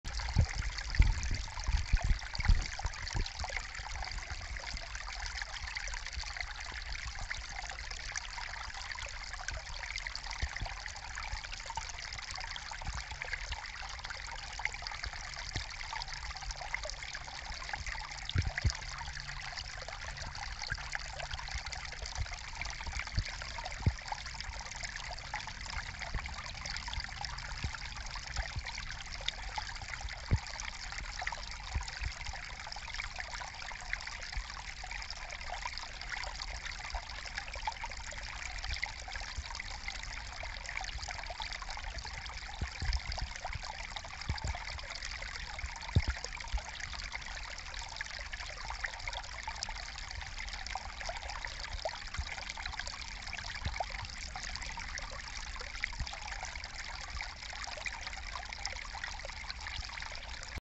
chill lo-fi brook sounds to relax / study to
(but only for 1 minute and also with some wind noises)